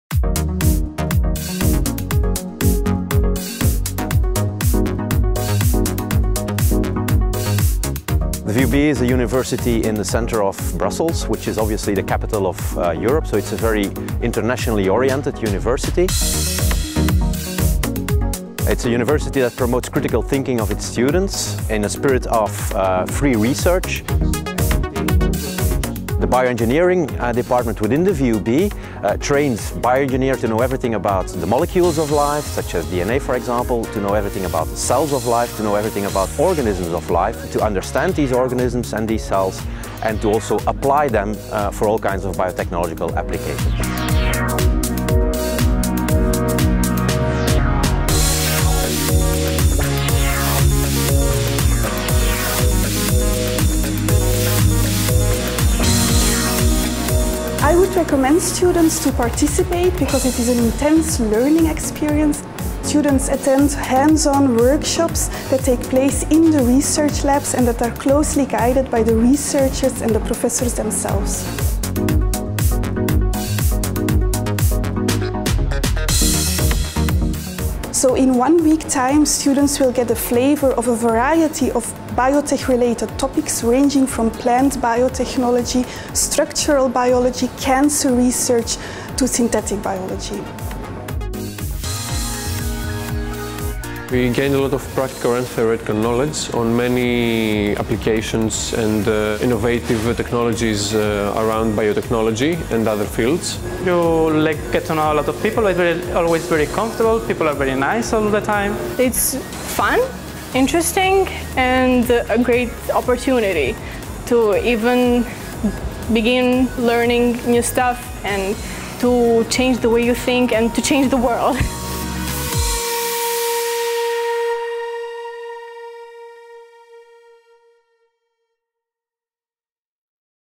VUB Summer School Biotech: Where Biology Meets Technology – Join us in Brussels!
• Are you a bachelor student with a background in biology, chemistry, biotechnology or biomedical sciences?
• Do you want to make a head start in your education and learn more about topics related to cancer research, synthetic biology, structural biology, plant genetics, microbial biotechnology and research valorization?
• Do you want to learn how real-world applications are developed from biotechnological research?
Then join us in Brussels for our 2-week Biotech Summer School